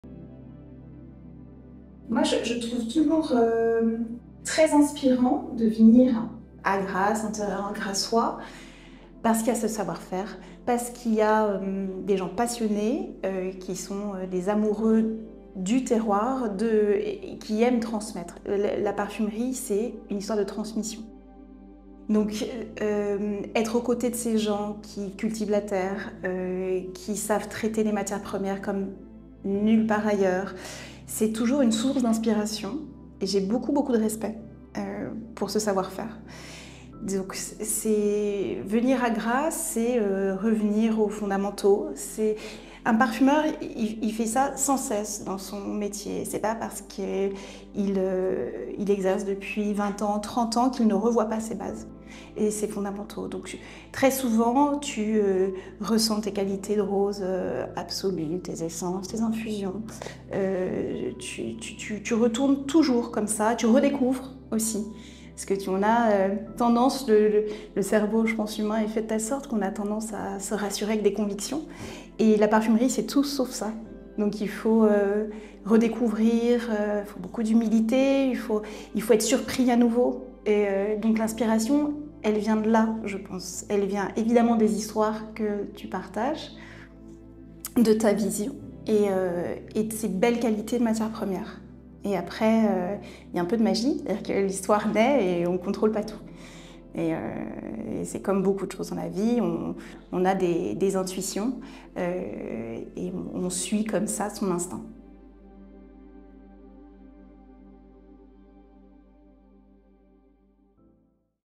Behind the Scent - Grasse, The Origins
"Perfumery is a story of transmission. Being surrounded by people who cultivate the land and work raw materials like no one else is always a source of inspiration. I have great respect for this know-how." - Marie Salamagne⁠